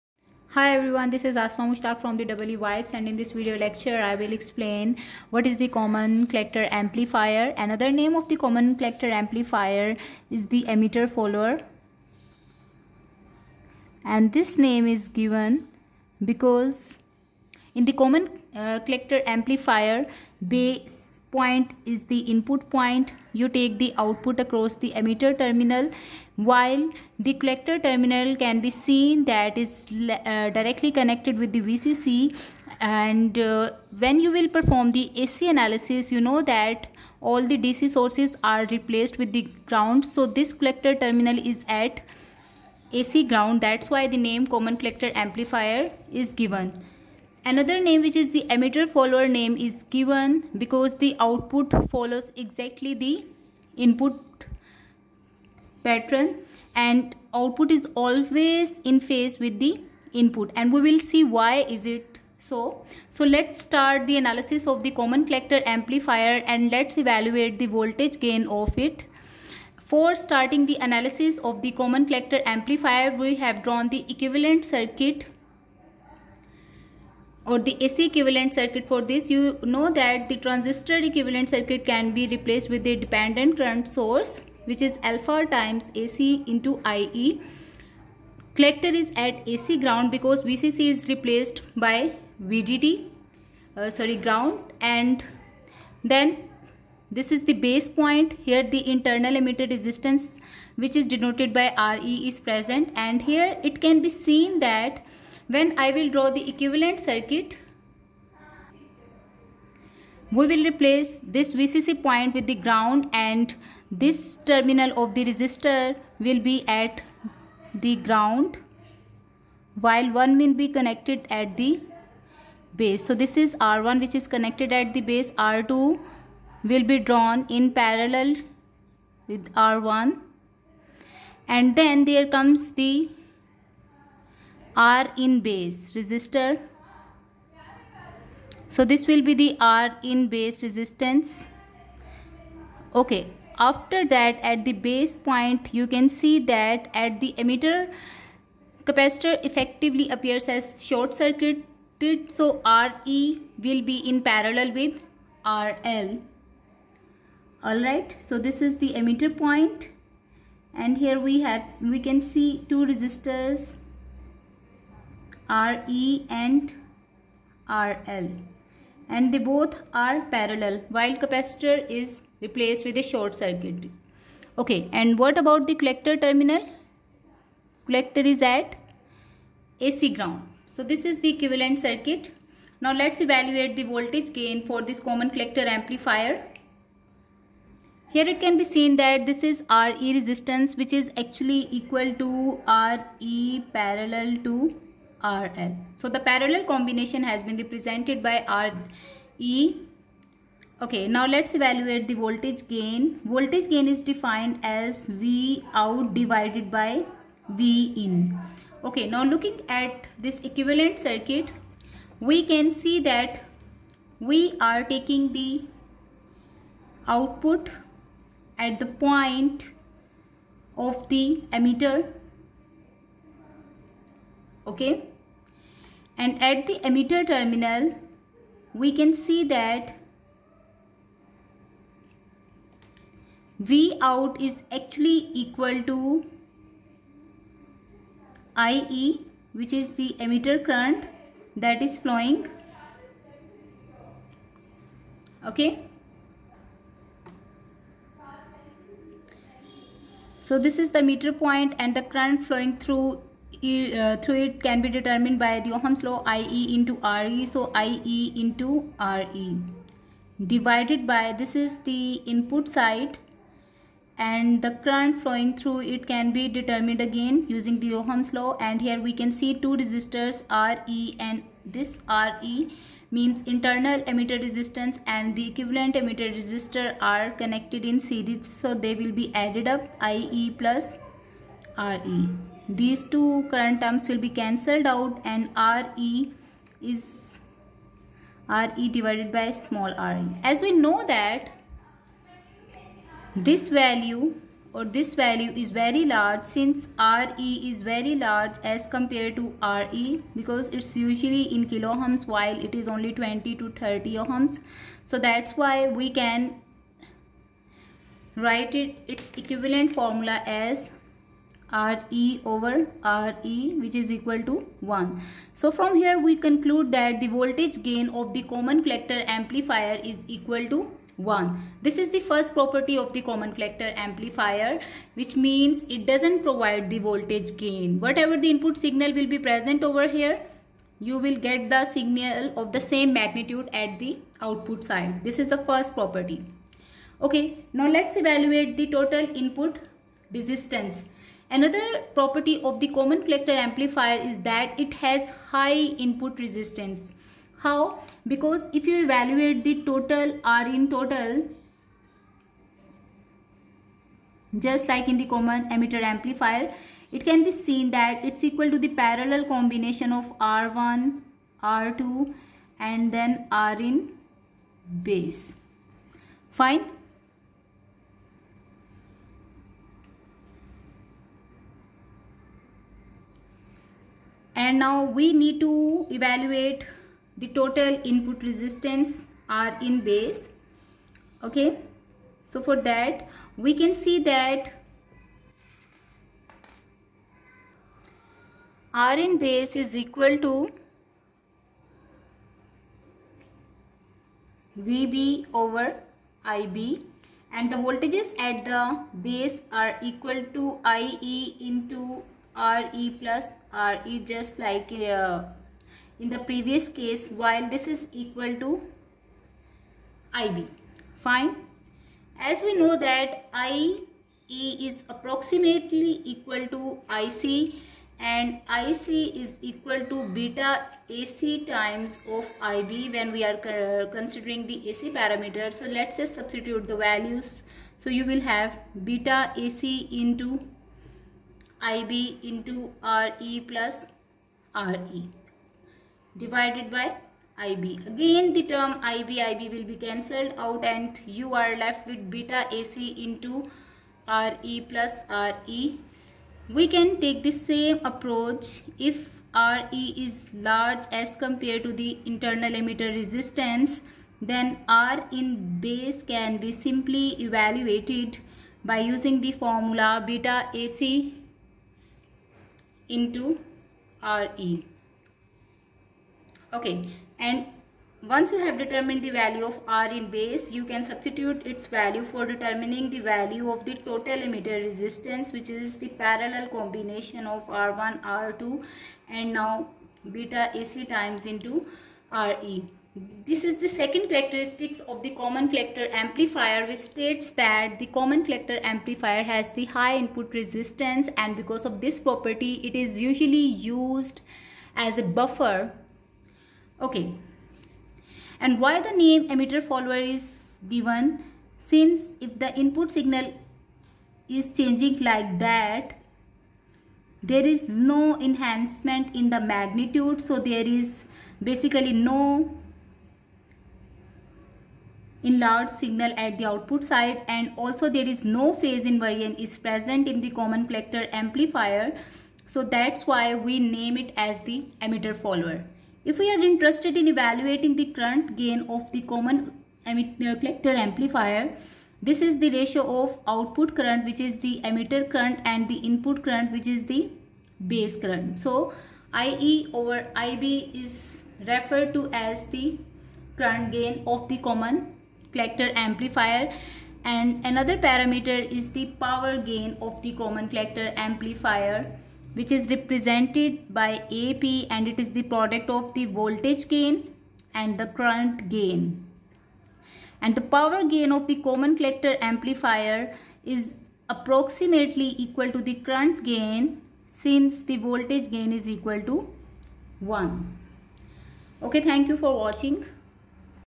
Common Collector Amplifier-Emitter Follower
Common Collector Amplifier is also known as emitter-follower. Since the output voltages are in phase with the input signal and the magnitude is almost same as the input. Common Collector amplifier has the characteristics of high input resistance and it provides unity voltage gain approximately (AV=1). It only provides the current gain which is the ratio of output current (Ie) and the input current (Ib).  The term common collector is used since the collector terminal is at ac ground since we replace all the voltage sources with ground while performing the ac analysis.